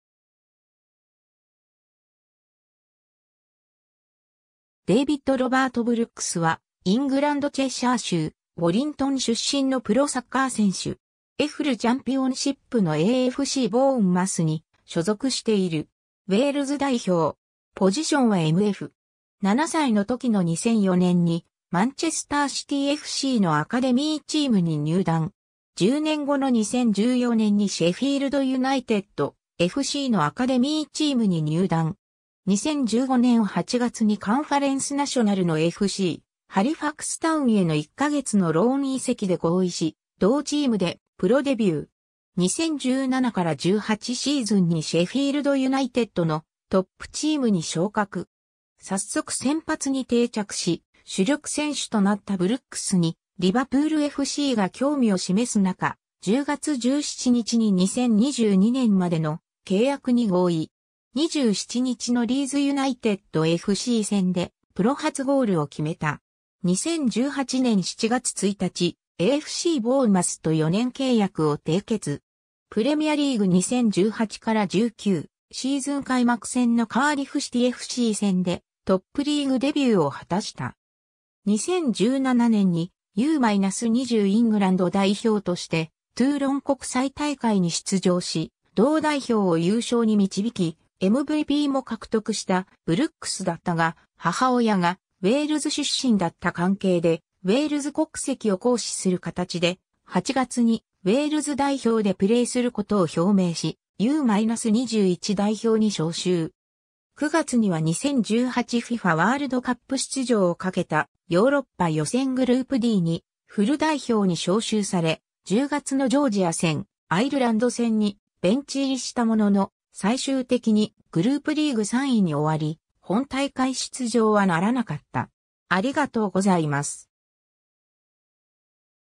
デイヴィッド・ブルックス
シェフィールド・ユナイテッドFCの選手
FCハリファクス・タウンの選手
デイヴィッド・ロバート・ブルックス（David Robert Brooks ., 1997年7月8日 - ）は、イングランド・チェシャー州ウォリントン出身のプロサッカー選手。
EFLチャンピオンシップのAFCボーンマスに所属している。
ウェールズ代表。
 7歳の時の2004年に、マンチェスター・シティFCのアカデミーチームに入団。
10年後の2014年にシェフィールド・ユナイテッドFCのアカデミーチームに入団。
2015年8月にカンファレンス・ナショナル (実質5部) のFCハリファクス・タウンへの1ヶ月のローン移籍で合意し、同チームでプロデビュー。
 2017-18シーズンにシェフィールド・ユナイテッドのトップチームに昇格。
早速先発に定着し、主力選手となったブルックスにリヴァプールFCが興味を示す中、10月17日に2022年までの契約に合意。
27日のリーズ・ユナイテッドFC戦で、プロ初ゴールを決めた。
 2018年7月1日、AFCボーンマスと4年契約を締結。
プレミアリーグ2018-19シーズン開幕戦のカーディフ・シティFC戦で、トップリーグデビューを果たした。
 2017年にU-20イングランド代表としてトゥーロン国際大会に出場し、同代表を優勝に導き、MVPも獲得したブルックスだったが、母親がウェールズ出身だった関係で、ウェールズ国籍を行使する形で、8月にウェールズ代表でプレーすることを表明し、U-21代表に招集。
9月には2018 FIFAワールドカップ出場を賭けたヨーロッパ予選グループDにフル代表に招集され、10月のジョージア戦、アイルランド戦にベンチ入りしたものの、最終的にグループリーグ3位に終わり、本大会出場はならなかった。